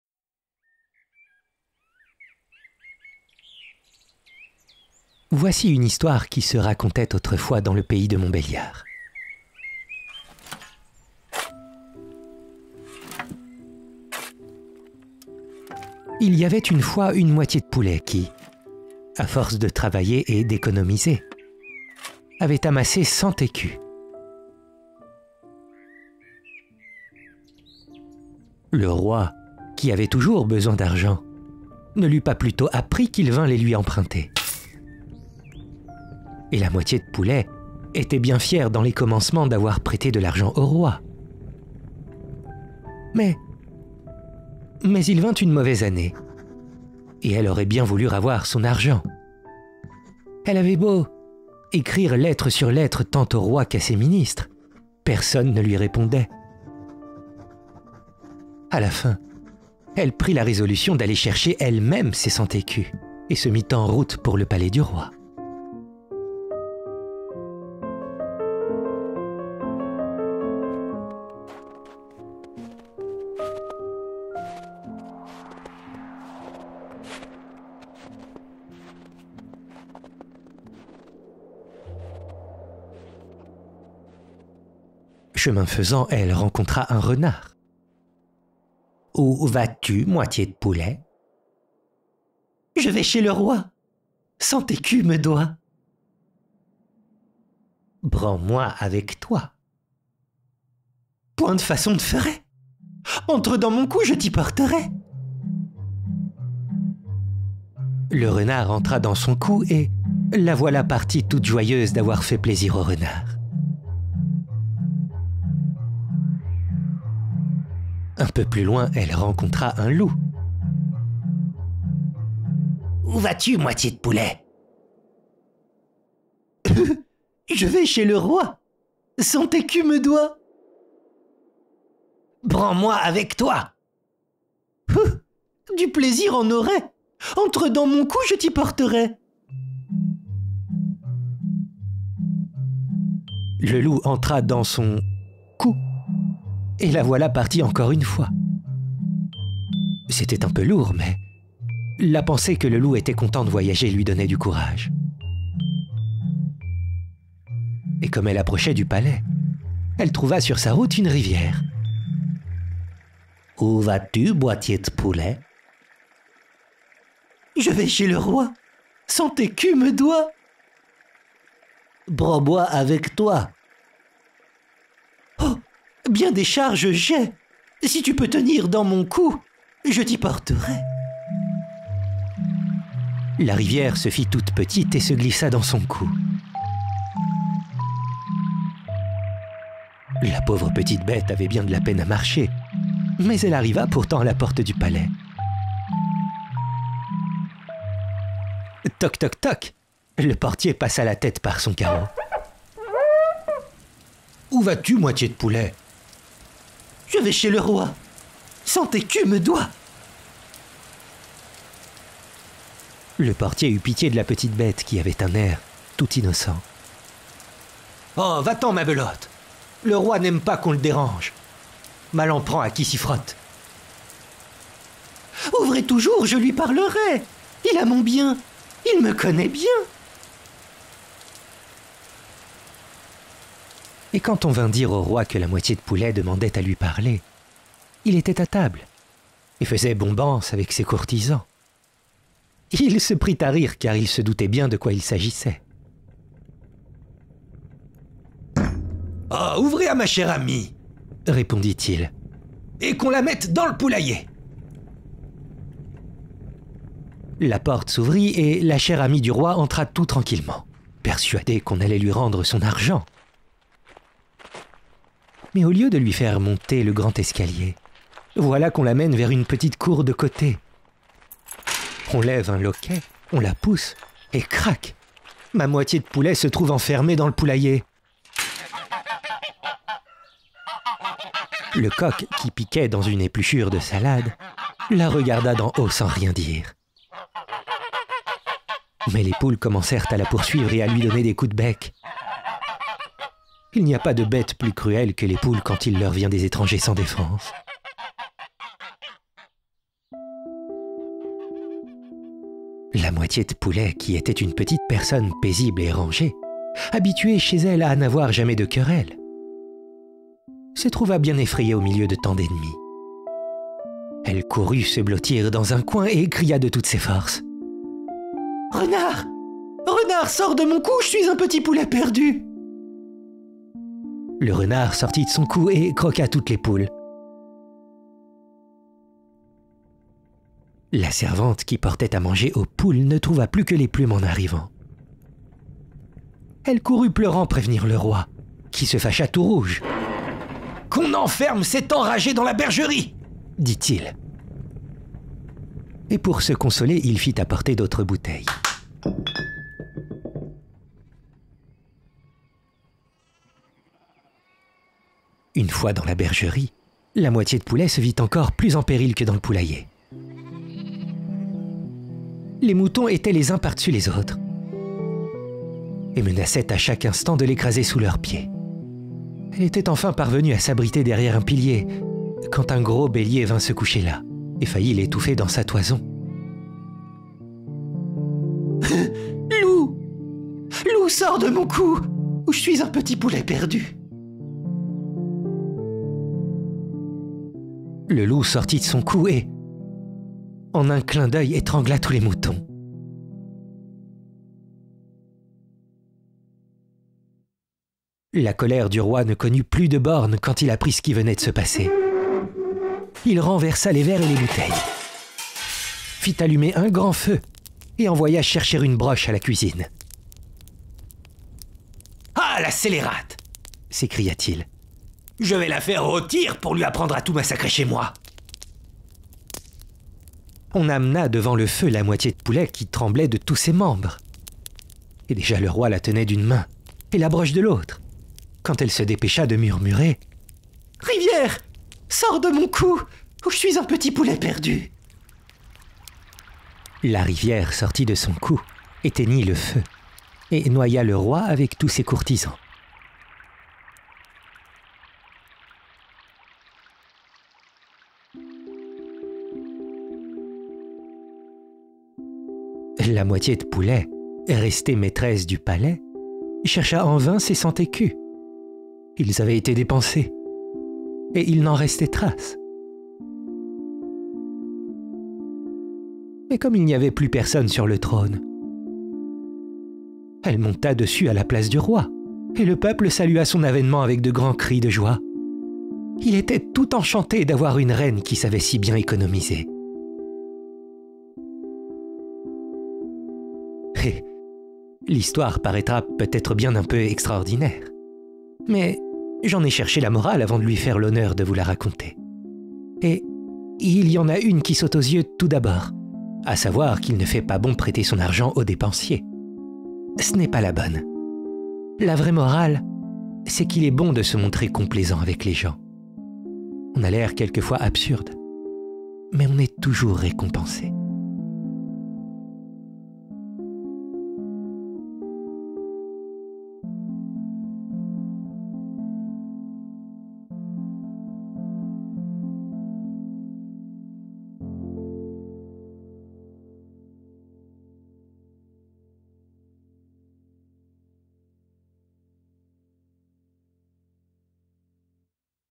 La Moitié de Poulet, Jean Macé (Conte pour Noël)
🦉Images générées par Midjourney
Lecture, musique et montage  : @Mojoon

La Moitié de Poulet, Contes du Petit-Château. Jean Macé. J. Hetzel, 1862, Paris.

En vous souhaitant une belle écoute !

NB : Pour une meilleure lecture,  je vous incite à vous nimber d'un casque :)